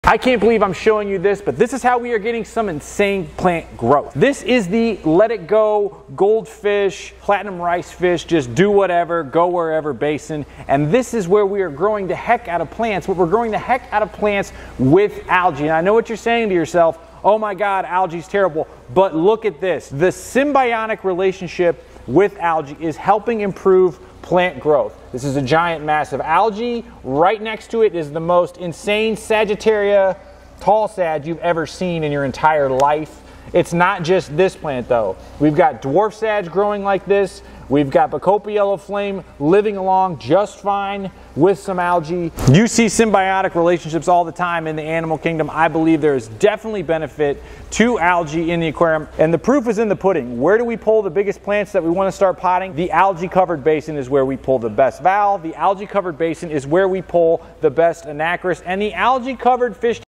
Symbiotic Relationship Between Algae And Plants!
I can't believe I'm showing you this, but this is how we are getting INSANE PLANT GROWTH! The symbiotic relationship between algae and aquarium plants is helping them grow wild! What are your thoughts on this? TANK ON! 👇👇👇